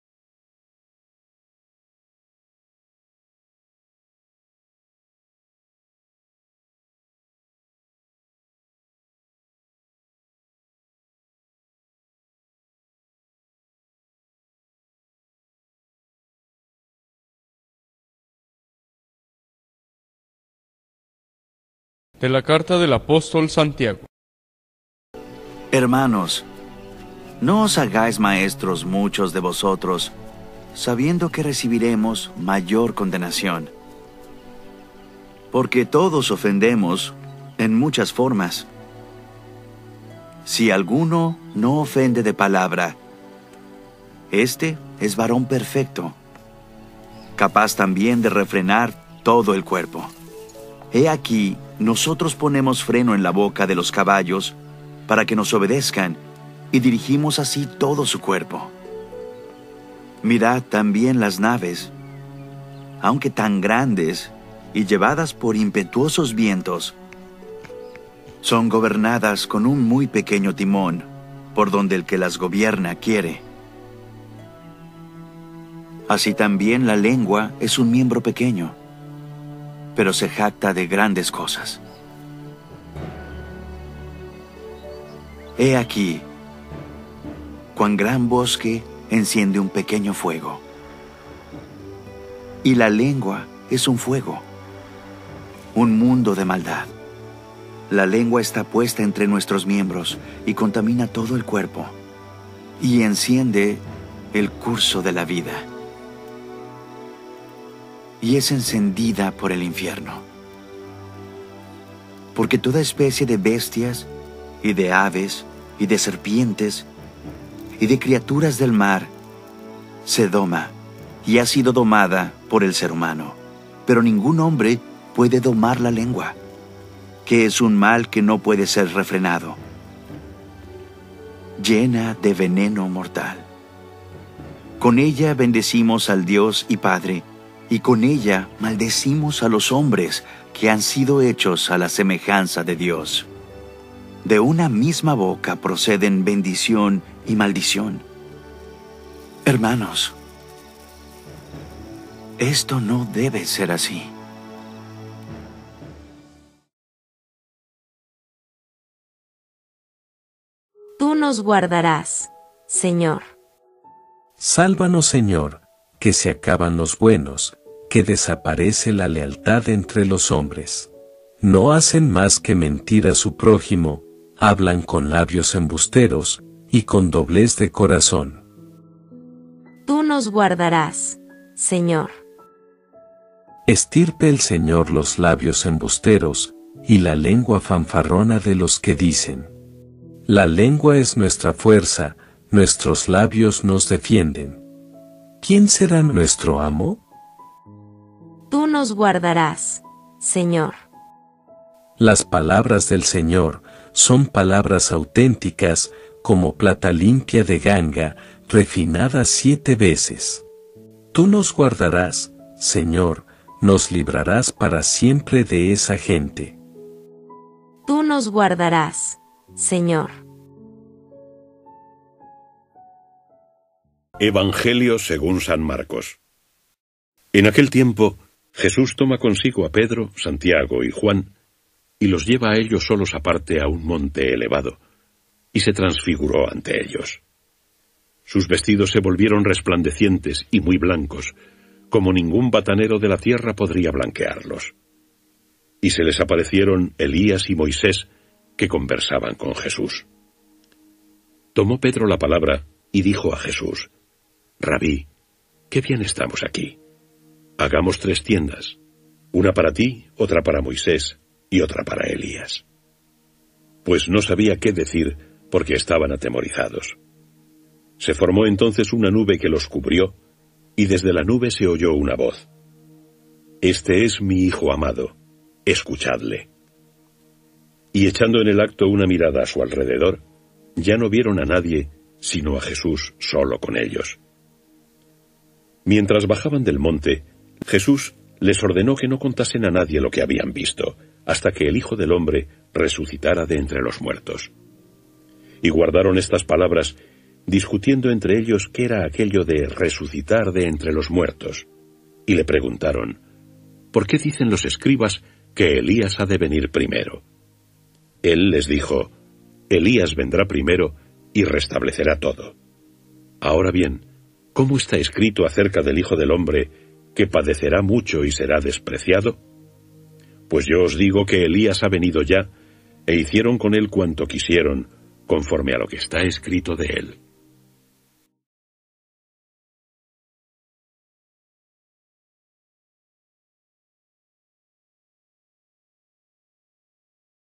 EVANGELIO DE HOY SÁBADO FEBRERO 19 DE 2022 | MARCOS 9,2-13
EVANGELIO Y LECTURAS EUCARISTÍA SÁBADO FEBRERO 19 DE 2022 SÁBADO SEXTA SEMANA TIEMPO ORDINARIO CICLO C Santiago 3, 1-10 Salmo 11, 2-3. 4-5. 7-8. R/. Tú nos guardarás, Señor. Marcos 9,2-13